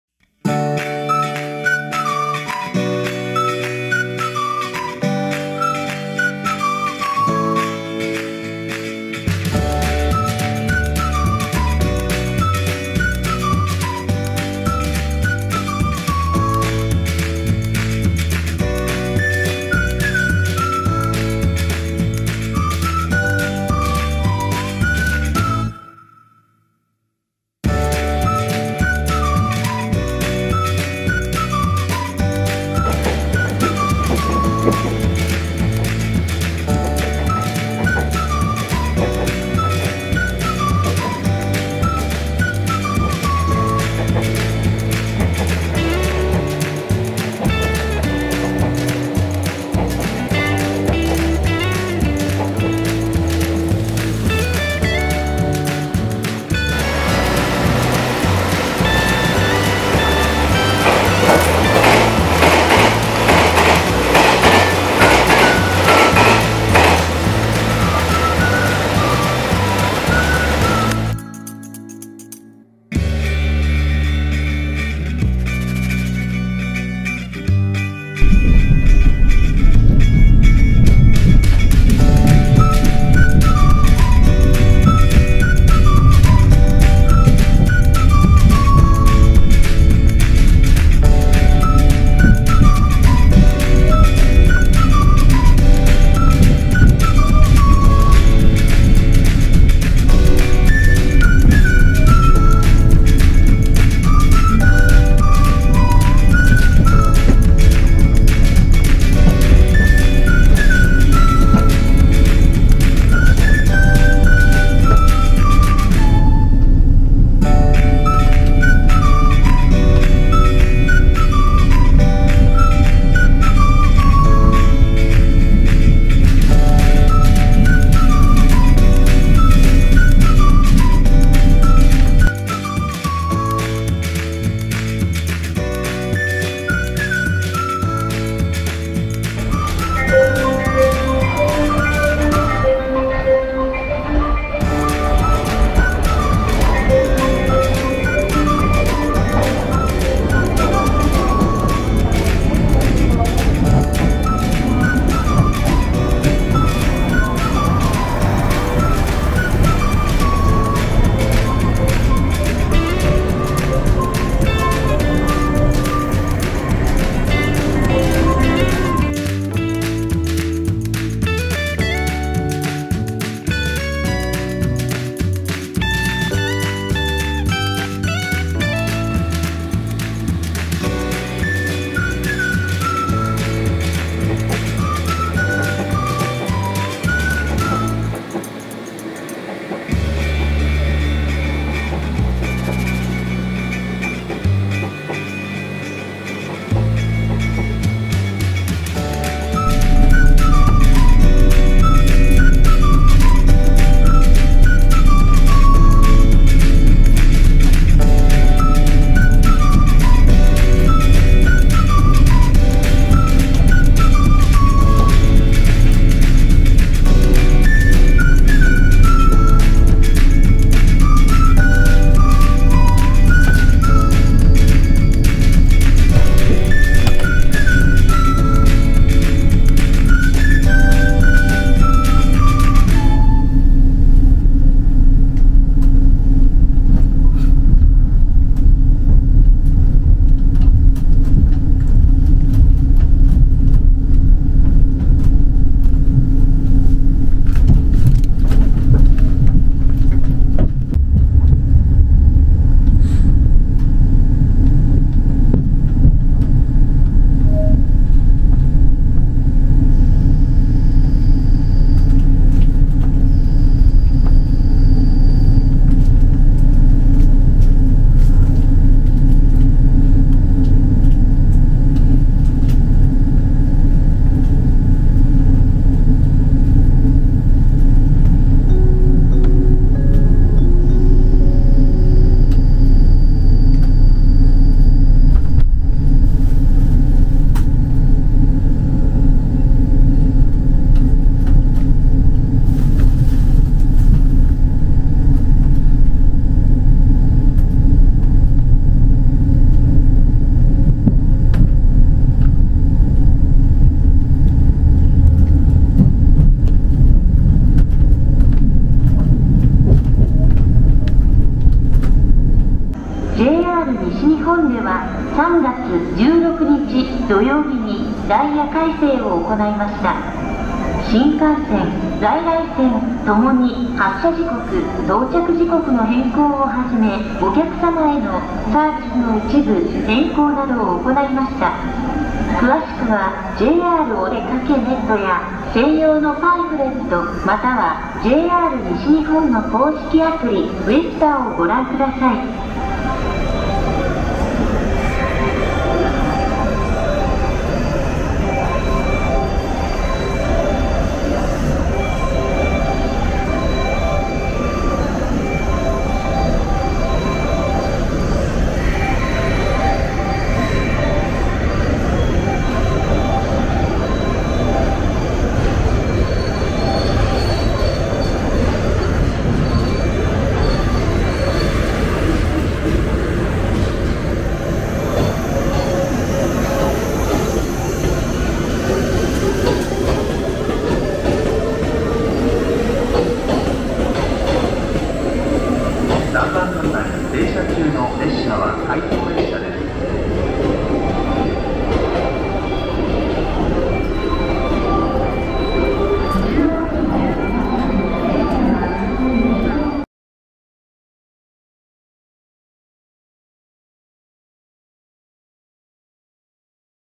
4色の381系やくも
2024年3月
引退間近の381系やくもを見に行ってきました。
リバイバル色と合わせて４色のやくもを見ることができました。